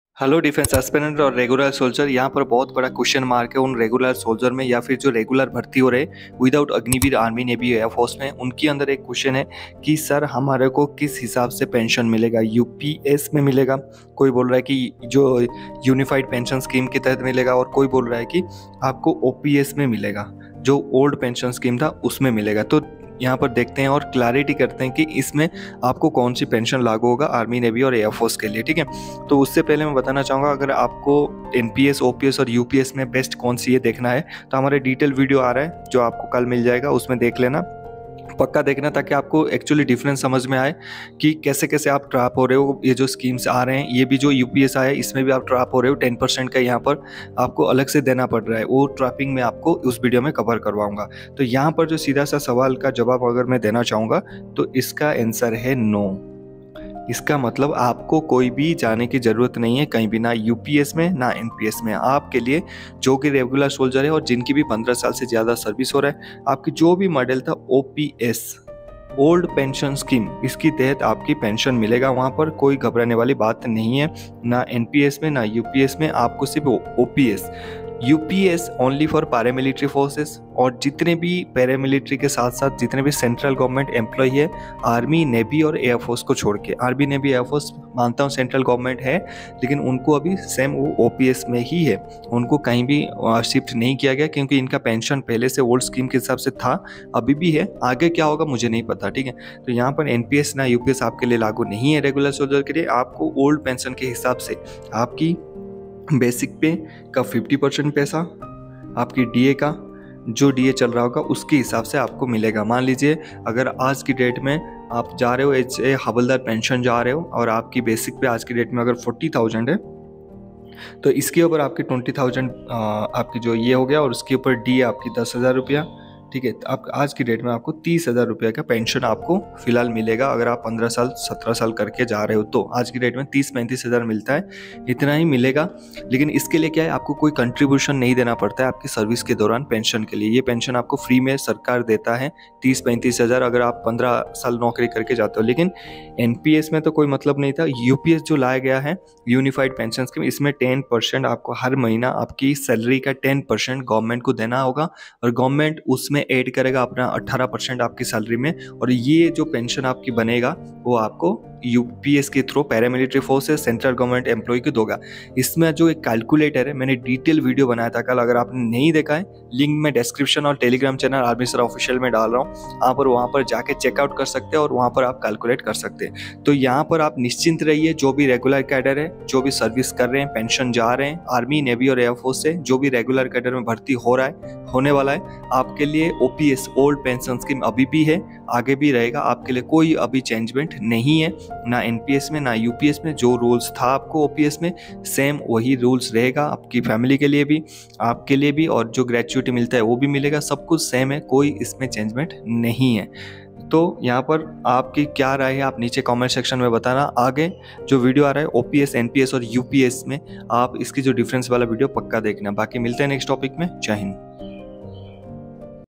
Army Navy Airforce Permanent Soldiers को Pension किस Scheme की तहत मिलेगा ? OPS ya UPS
Regular & Permanent Soldiers ki PENSION ka kya hoga?
OPS ya UPS